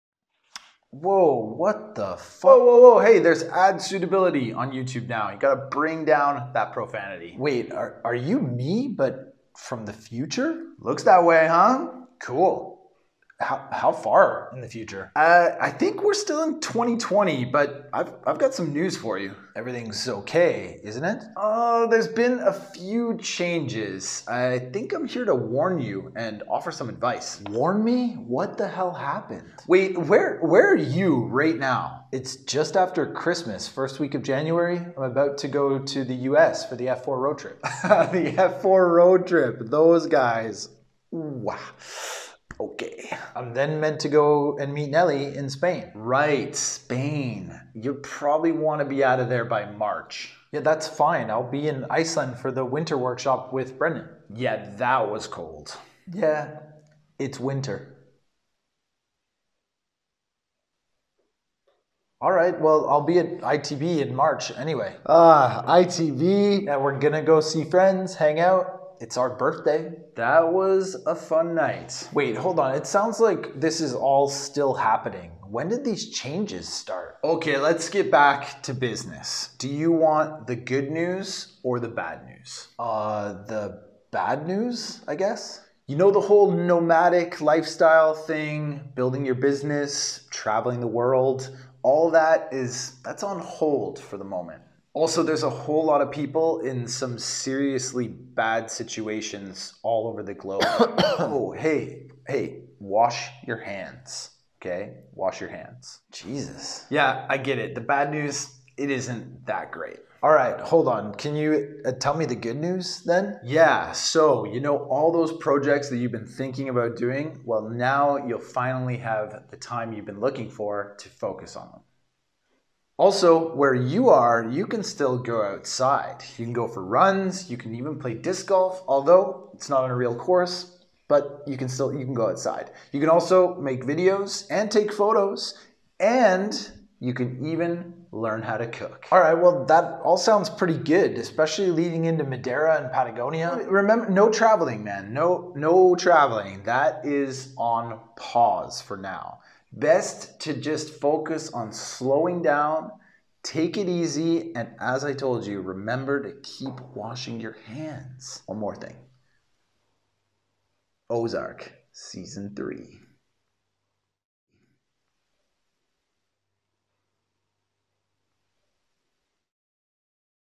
PANDEMIC & my PAST SELF Inspired by JULIE NOLKE
If you could talk to your past self right now, what would you say?

This weekend Nellie and I were talking about the genius video by Canadian comedian @julienolke where she is Explaining the Pandemic to her Past Self

We decided to creatively challenge ourselves to do the same. This is my attempt at explaining the pandemic to my past self from future me. It was of course more difficult than we thought and overall a really fun process to create. Shout out to @NellieWinter & Julie Nolke for the awesome inspiration :)